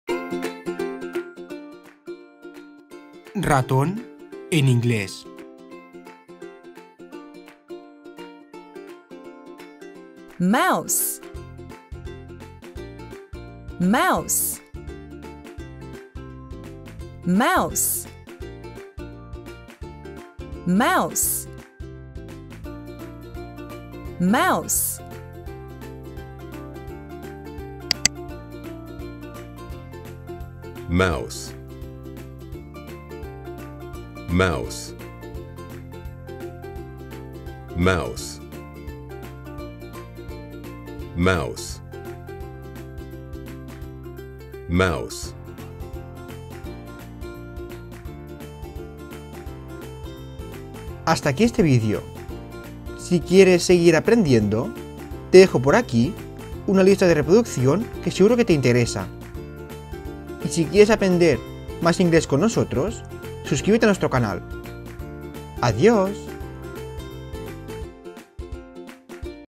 ✅ CÓMO se dice RATÓN en INGLÉS 2022 🚀 APRENDE A ESCRIBIR MOUSE ✨ PRONUNCIACIÓN RATONES INGLES 2023
▶️ Aprende a CÓMO decir y escribir RATÓN correctamente en inglés ✅

Introducción: 00:00
Pronunciación en femenino: 00:08
Suscríbete: 00:25
Pronunciación en masculino: 00:29
Lista de reproducción recomendada: 00:49